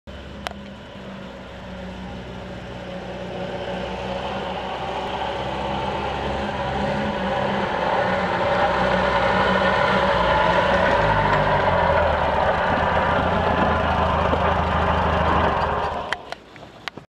Opel Kübelwagen kommt mit über 60 Jahre Verspätung an
Gerade noch rechtzeitig zur Finanzkrise 2012 kommt Feldwebel Deck stark angeschlagen mit seinem Kübelwagen wieder in der Heimat an.